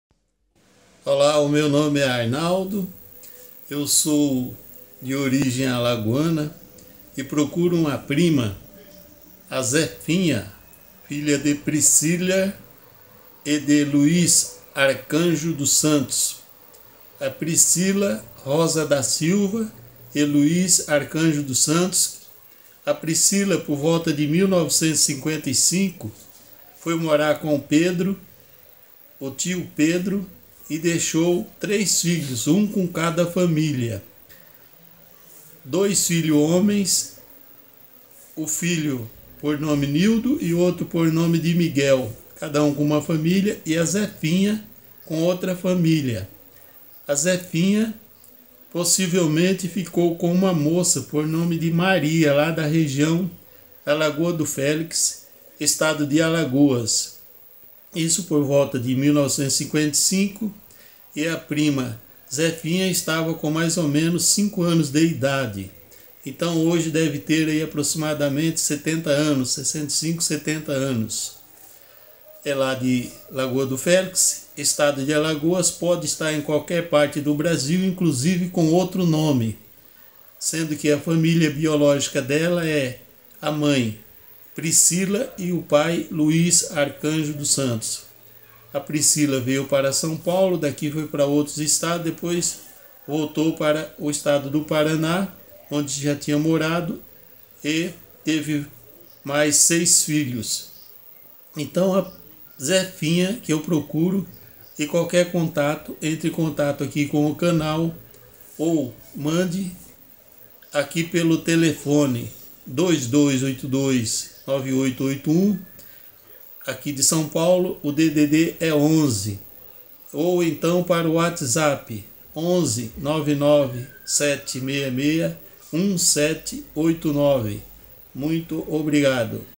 QUEREMOS ENCONTRAR ZEFINHA ARCANJO DOS SANTOS ALAGOANA DE IGACI OU DA REGIÃO DE ARAPIRACA ALAGOAS
A FOTO DE CAPA É DE VALDEVINO MEIO IRMÃO DE ZEFINHA
ZEFINHA É FILHA BIOLÓGICA DE PRISCILIA ROSA DA SILVA MAIS CONHECIDA POR CAÇULA E DE LUIS ARCANJO DOS SANTOS
FOI DEIXADA PELA MÃE EM 1955 APROXIMADAMENTE COM ALGUÉM
NÃO SABEMOS COM QUEM SE COM UMA FAMÍLIA OU COM UMA MOÇA
SABE-SE QUE FICOU NA REGIÃO DE ARAPIRACA ESTADO DE ALAGOAS
PRÓXIMO AO POVOADO LAGOA DO FÉLIX MUNICÍPIO DE IGACI
PEDE-SE A QUEM TIVER QUALQUER TIPO DE INFORMAÇÃO ENTRAR EM CONTATO COM ESSE CANAL OU COM OS TELEFONES OU PÊLO WHATSAPP
A FAMÍLIA DESDE JÁ AGRADECE